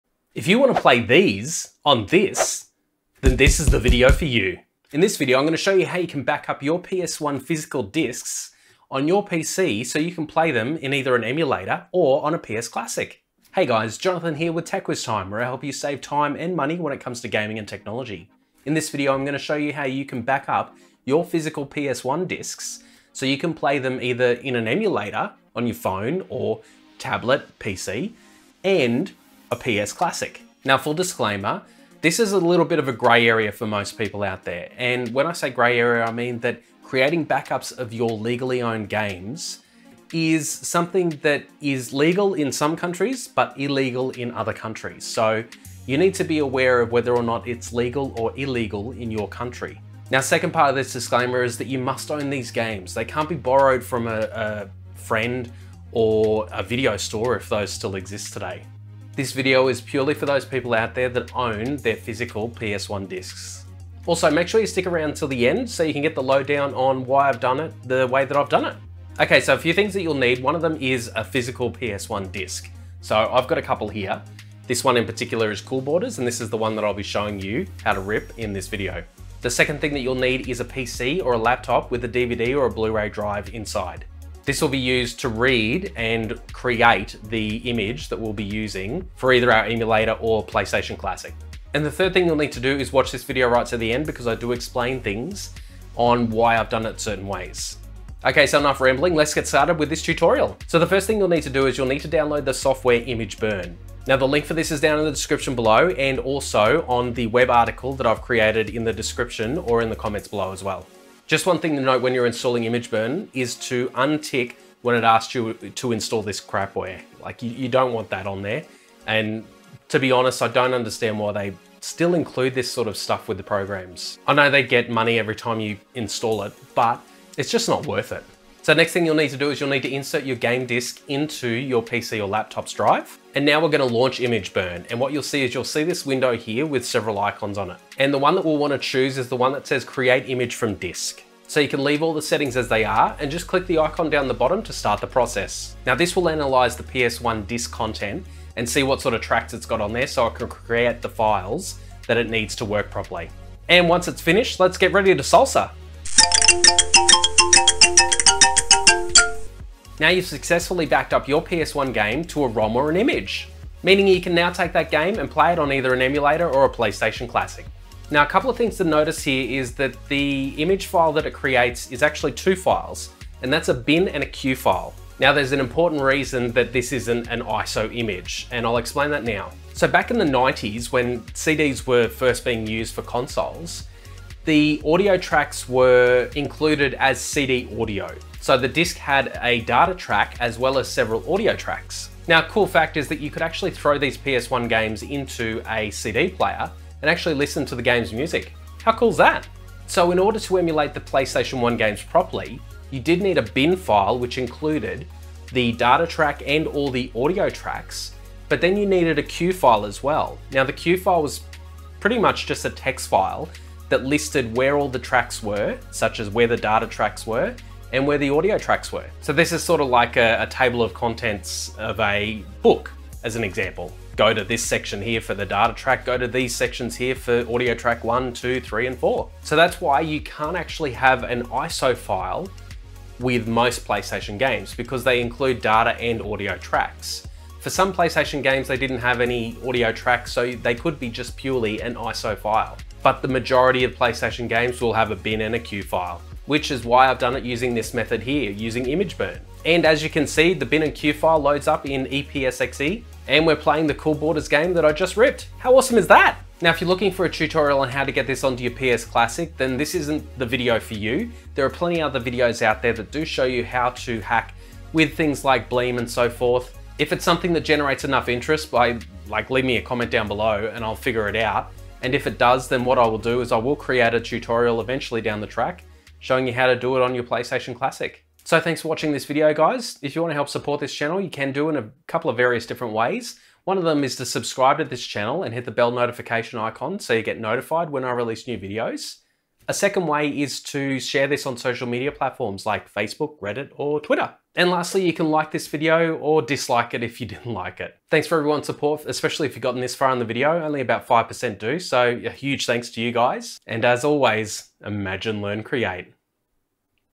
Backup PS1 Games on PC for PS Classic or Emulation - FREE SOFTWARE
If you are looking at using your physical Playstation Games on a PS Classic or emulator like Epsxe, then this video will teach you how to backup your discs the right way. Very simple process and best of all, it's FREE!

Web Article - Coming Soon